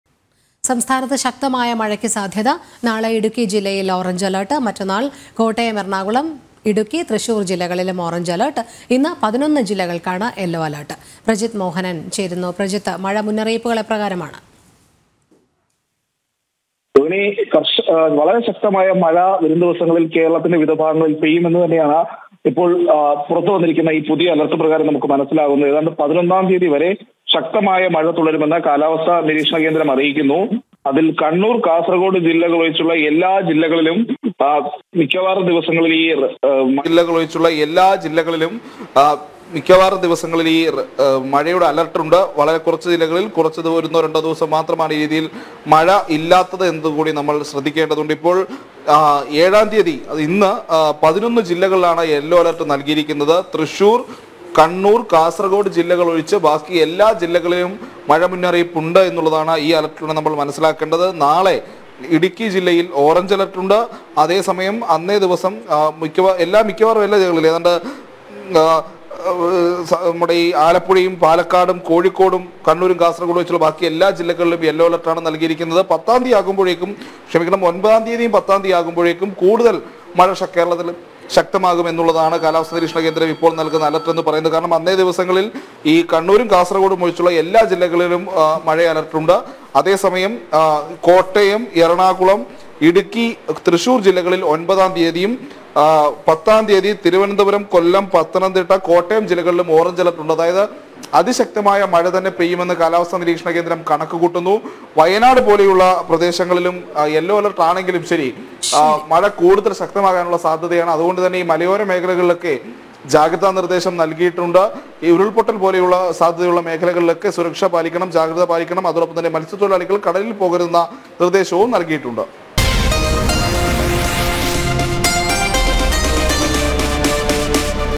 സംസ്ഥാനത്ത് ശക്തമായ മഴക്ക് സാധ്യ
സംസ്ഥാനത്ത് ശക്തമായ മഴക്ക് സാധ്യത. നാളെ ഇടുക്കി ജില്ലയിൽ ഓറഞ്ച് അലർട്ട്. മറ്റെന്നാൾ കോട്ടയം, എറണാകുളം, ഇടുക്കി, തൃശൂർ ജില്ലകളിലും ഓറഞ്ച് അലർട്ട് പ്രഖ്യാപിച്ചു. ഇന്ന് 11 ജില്ലകൾക്ക് യെല്ലോ അലർട്ട്.